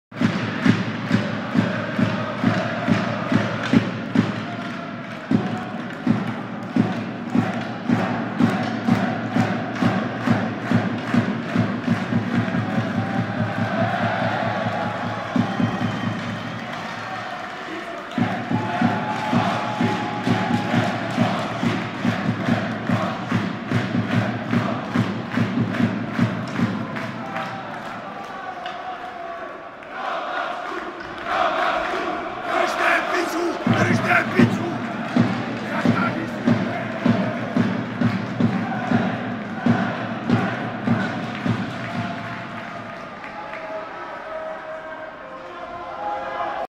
Konec utkání HC Olomouc HC Vítkovice 0:1 3.12.2017
Branka a nahrávka: 44. Olesz (Szturc). Rozhodčí: Hradil, Souček - Gebauer, Lhotský. Vyloučení: 5:5. Bez využití. V oslabení: 0:1. Diváci: 4207.